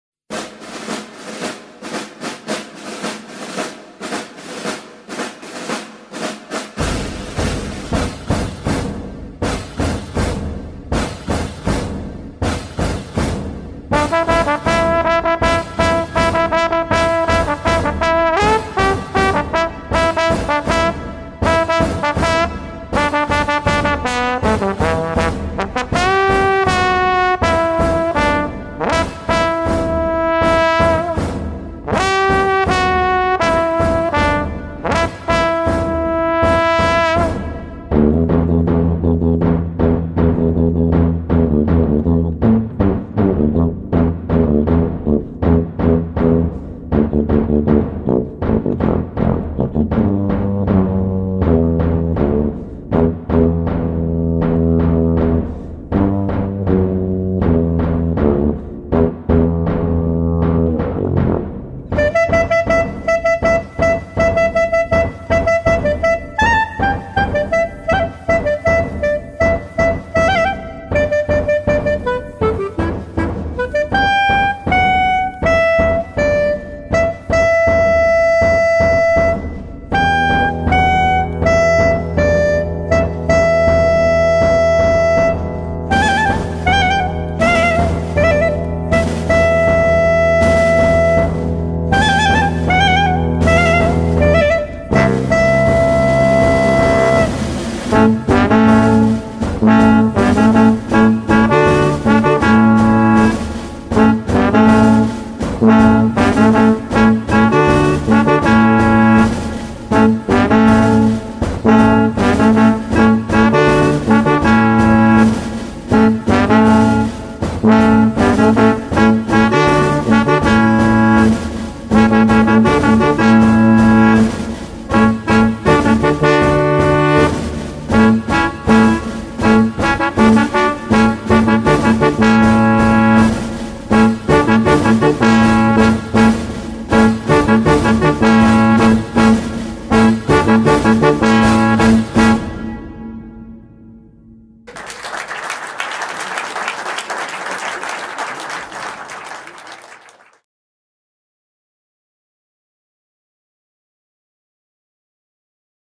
DIE PUNTE "a tribute to Freddie Mercury" @ Vienna, Nov. 2016
DIE PUNTE "a tribute to Freddie Mercury" on the 25th anniversary of his death feat. Werner Puntigam (trombone), Günther Schmidhuber (clarinet), H. G. Gutternigg (tuba) at Sargfabrik Vienna (A) on November 29, 2016.

(video recording & edit by Werner Puntigam / pntgm EAR X EYE)